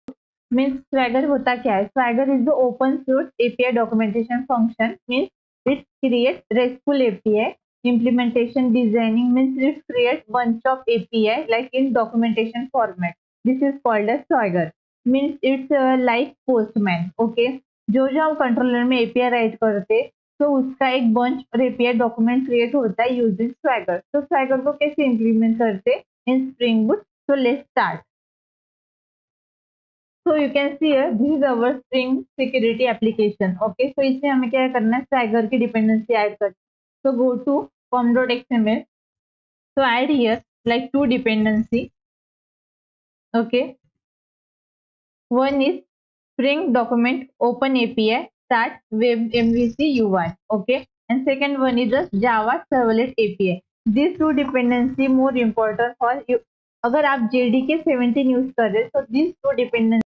Integrating Swagger with Spring Boot.🔔 Don't forget to like, comment, and subscribe
Description:
Swagger is a powerful tool for documenting and interacting with RESTful APIs. In Spring Boot, integrating Swagger is straightforward and provides a user-friendly interface to explore and test API endpoints. With Swagger, you can automatically generate detailed API documentation, complete with endpoint descriptions, parameters, and response types, enhancing both development and user experience.